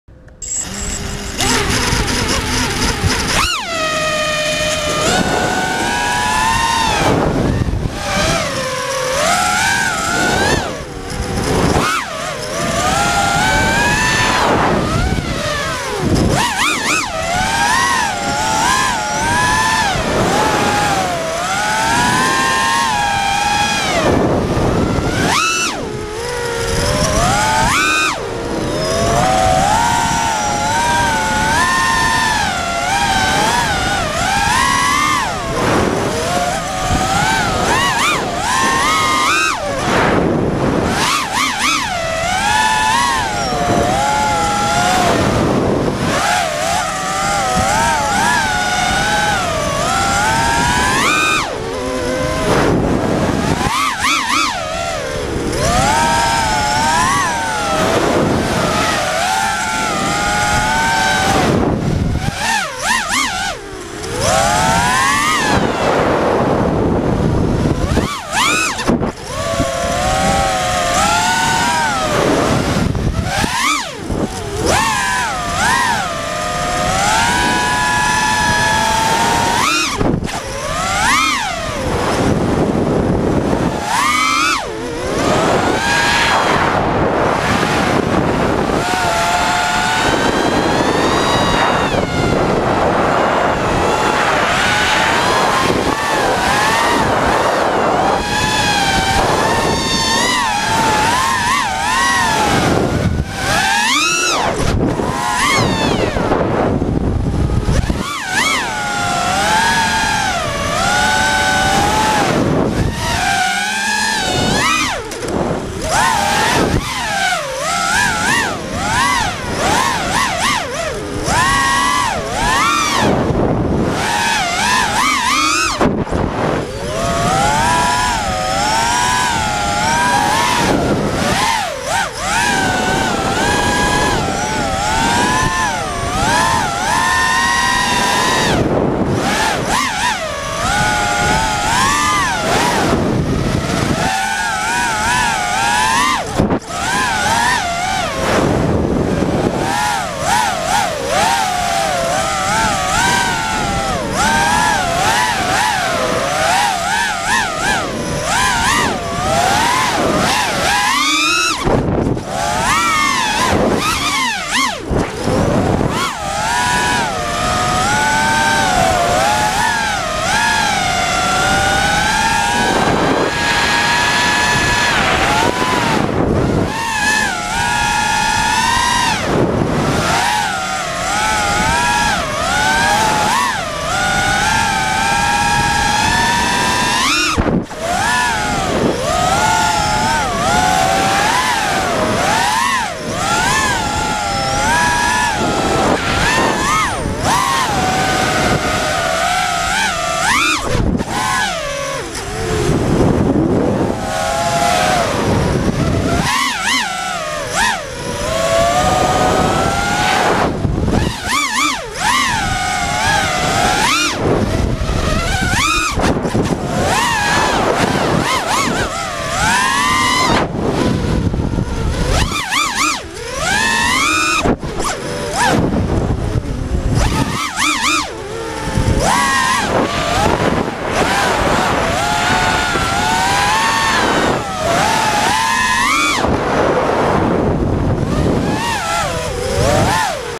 Stanford Rippin' HQ 5x4x4 - RAW
Testing the new HQ 5x4x4 from getpropsman.

Can you spot a boy on fire?
Fireboy and I got caught rippin' around Stanford; cops were jelly of our skills so we got kicked...

Setup:
ImpulseRc 5" Alien
Cobra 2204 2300kv (one is lightly crustified of the magnet variety)
5x4x4 props
Kiss FC, best FC ever.
HS1177 for FPV
GoPro 3+ (I'll be getting the 5 or 4+ when it is released)
Lumenier 1300 4s 60c
Little Bee 20 (Smokey killed my family for using Kiss 18a)
Lumenier TX5G6 600mw fpv tx

Leave a thumbs up or a sub if you think this is cool.